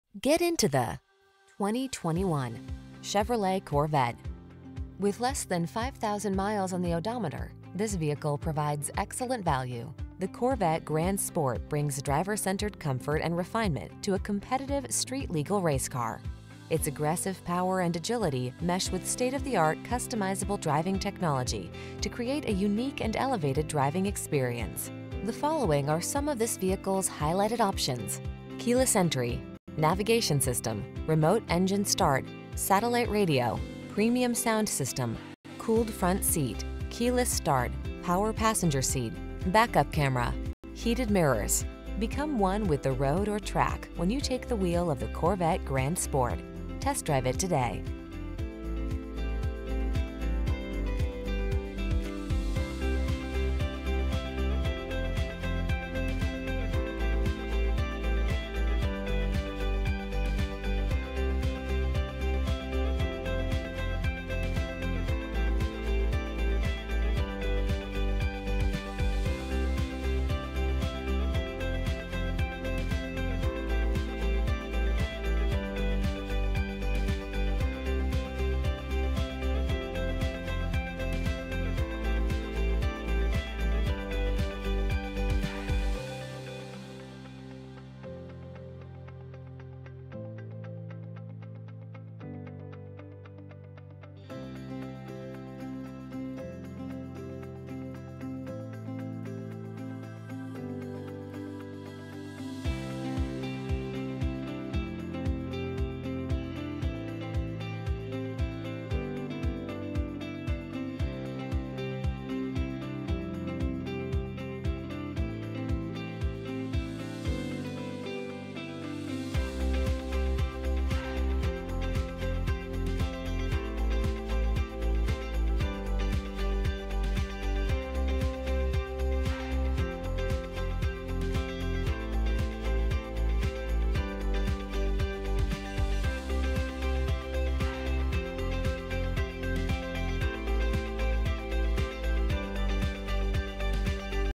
2021 Chevrolet Corvette Carson City, Reno, Yerington, Northern Nevada, Elko, NV P16169
Sebring Orange Tintcoat Used 2021 Chevrolet Corvette available in Carson City, Nevada at Champion Chevrolet Reno. Servicing the Reno, Yerington, Northern Nevada, Elko, NV area.